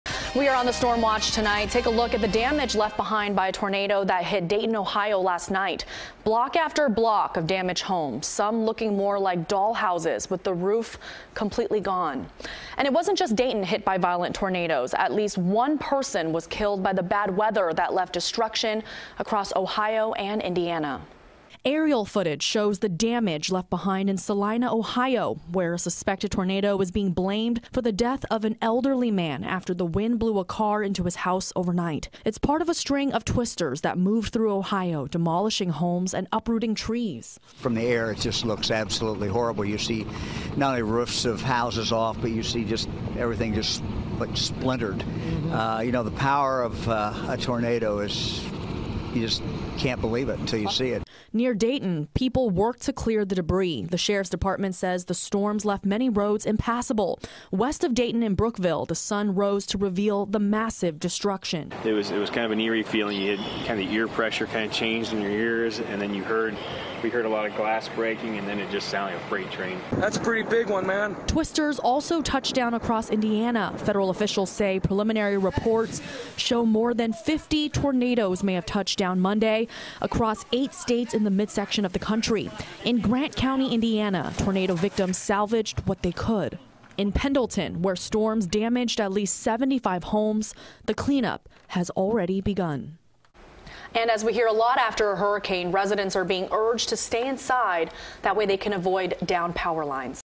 Deadly Tornadoes Hit Ohio
Violent tornadoes left devastation in Ohio and Indiana.